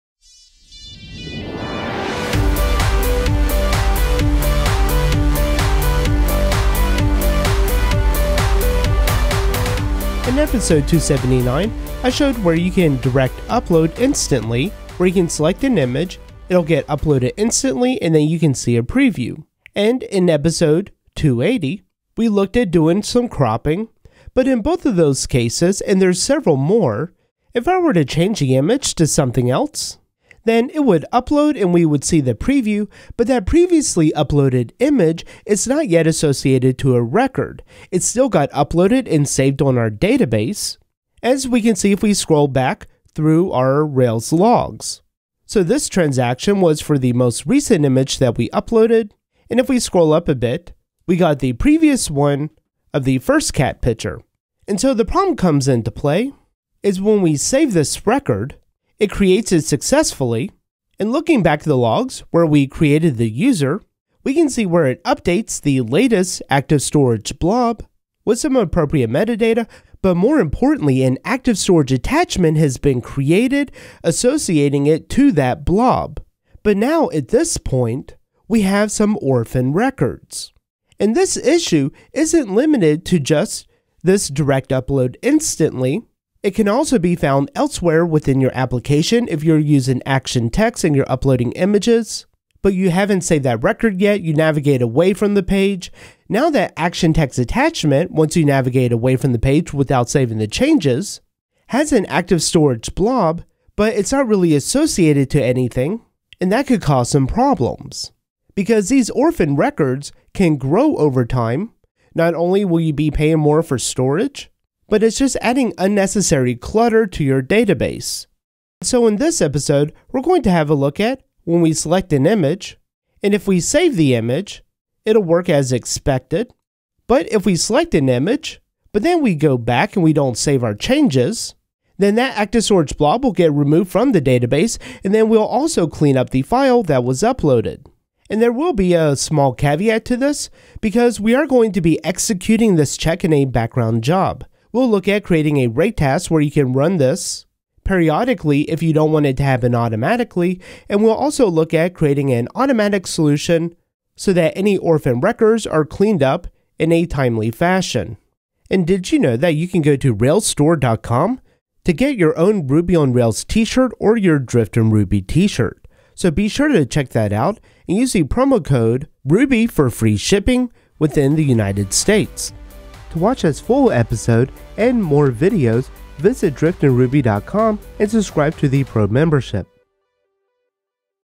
Episode #281 - Purge Orphaned Active Storage Records | Preview
Over time, orphaned records can build up within Active Storage. In this episode, we look at a few approaches to clean up these orphaned records.